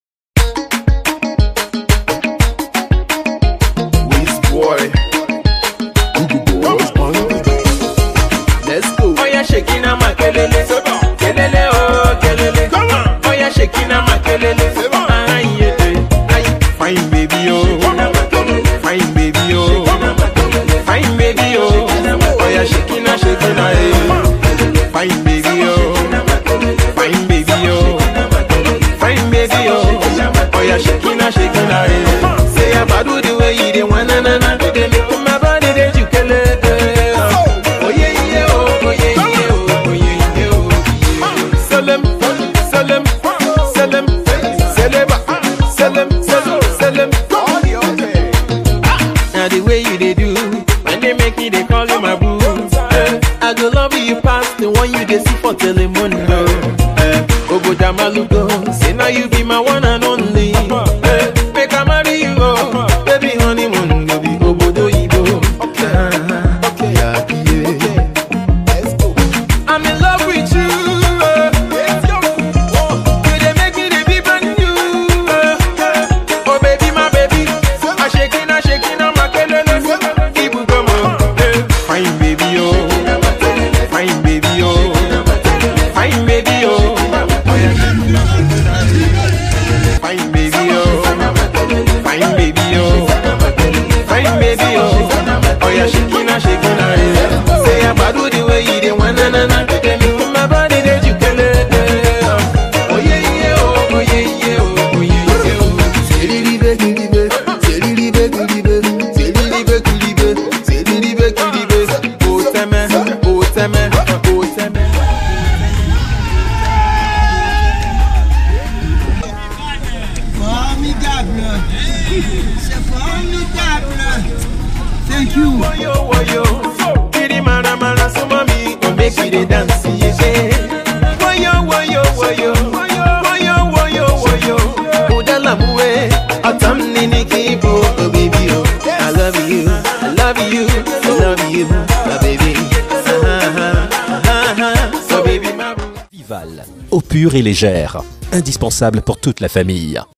Carna-Vival Brazzaville
YES WE CAN ! Suivez le récap du Carna-Vival zone sud, Brazzaville a encore été vivalement Vivalisé ce samédi 15 février.